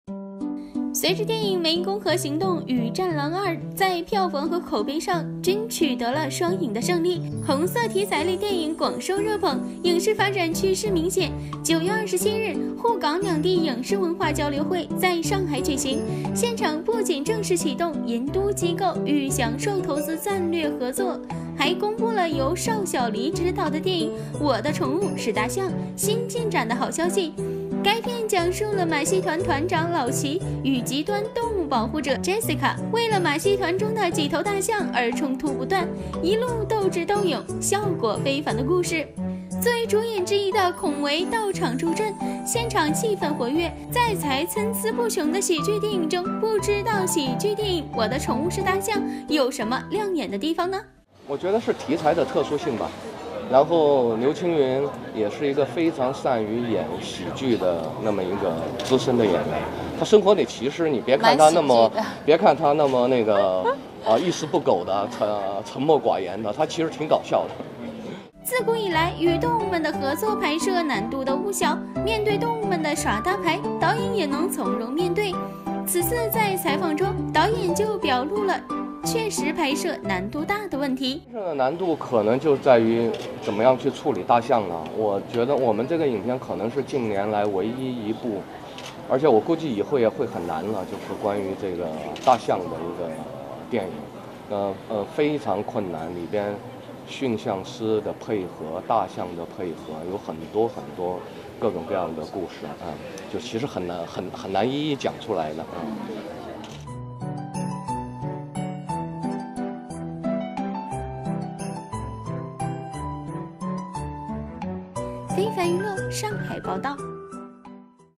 【2年前】滬港兩地影視文化交流會舉辦 邵曉黎吐露新電影拍攝難度大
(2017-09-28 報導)

隨著電影《湄公河行動》與《戰狼2》在票房和口碑上均取得了雙贏的勝利，紅色題材類電影廣受熱捧，影視發展趨勢明顯，9月27日滬港兩地影視文化交流會在上海舉行。